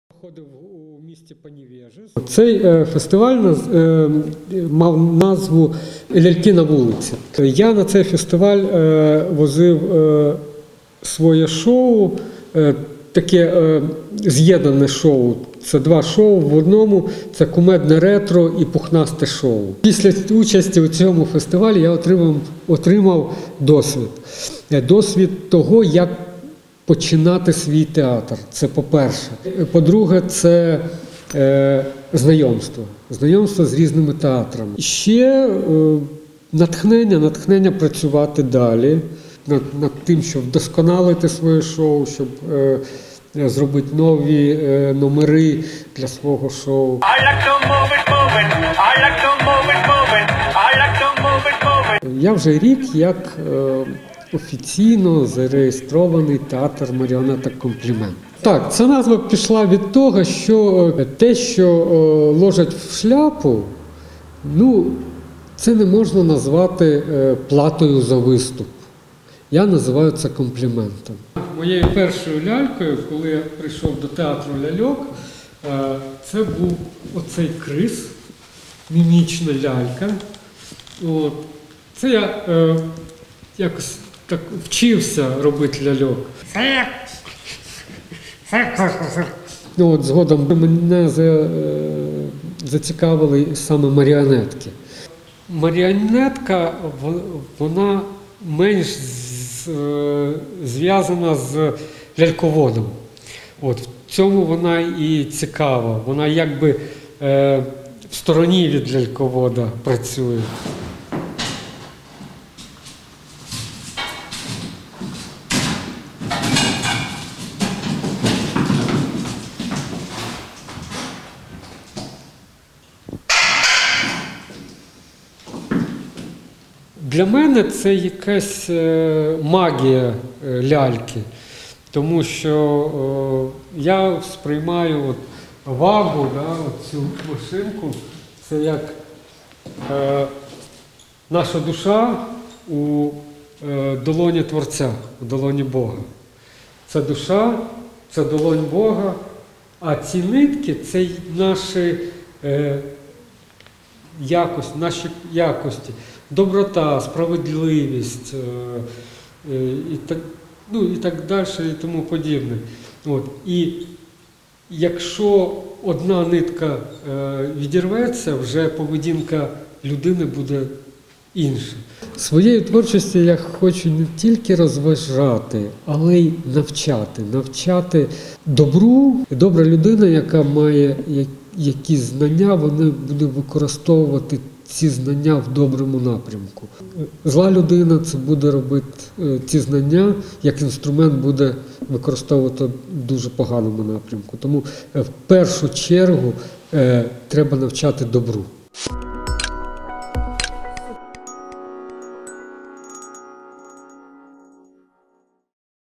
Перемога житомирського лялькаря Сергія Соловйова на Всесвітньому фестивалю вуличних театрів ляльок
Житомирський лялькар Сергій Соловйов та його театр маріонеток "СОMPLIMENT" здобули звання лауреата Всесвітнього фестивалю вуличних театрів ляльок, який проходив з 5 по 13 вересня у литовському місті Паневежисі.

На фестивалі було представлено 15 театрів з 8 країн Європи